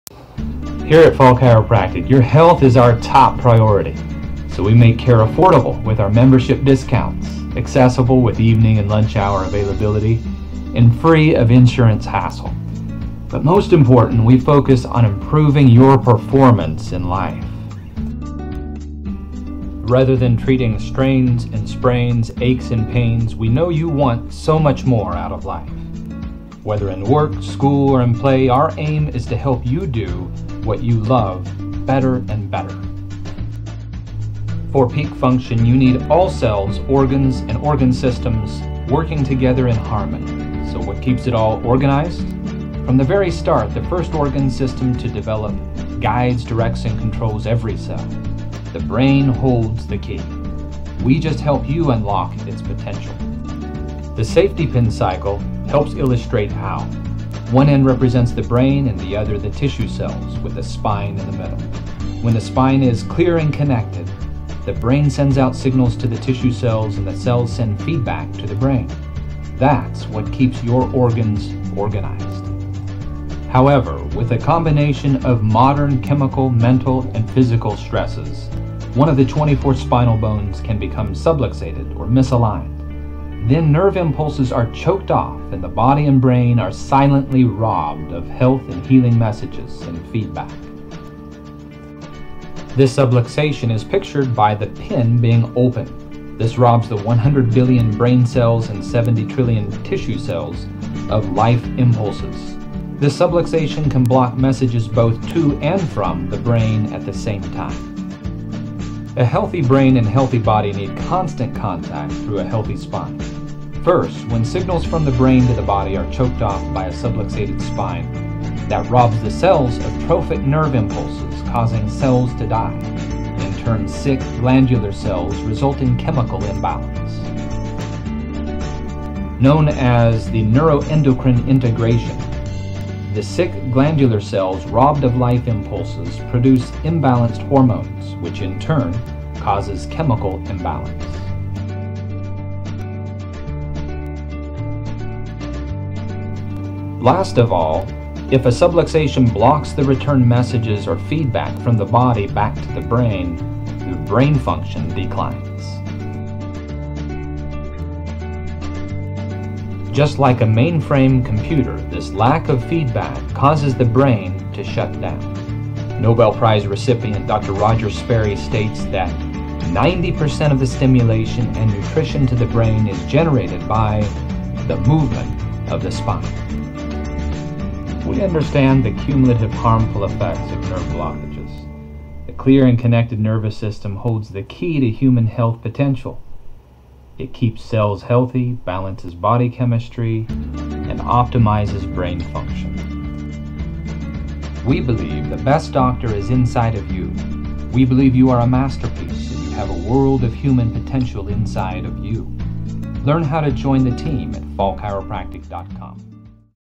What Is a Subluxation?
We share some of the more accepted pathways whereby a subluxation can silently be robbing your body of life and enhanced living.